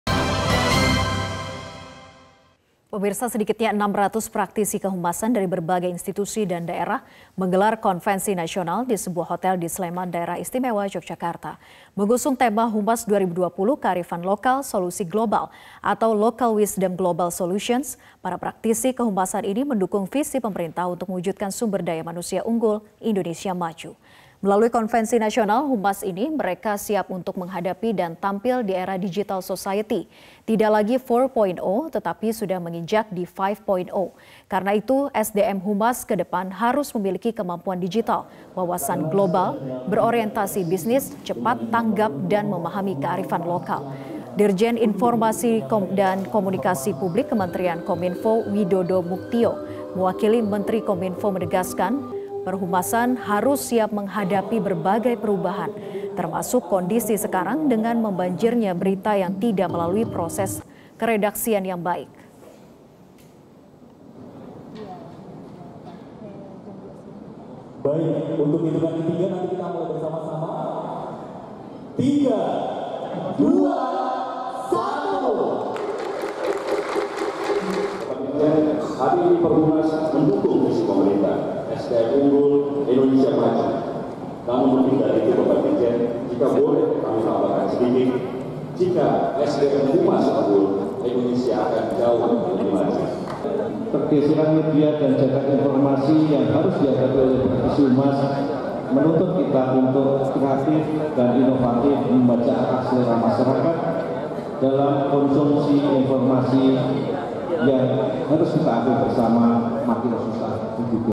Ratusan Praktisi Kehumasan Dukung Visi Pemerintah
600 praktisi kehumasan dari berbagai institusi dan daerah, menggelar konfrensi nasional sebuah hotel di Sleman, DIY. Mengusung tema humas 2020, "Kearifan Lokal Solusi Global", para praktisi kehumasan mendukung visi pemerintah untuk mewujudkan "Sumber Daya Manusia Unggul, Indonesia Maju".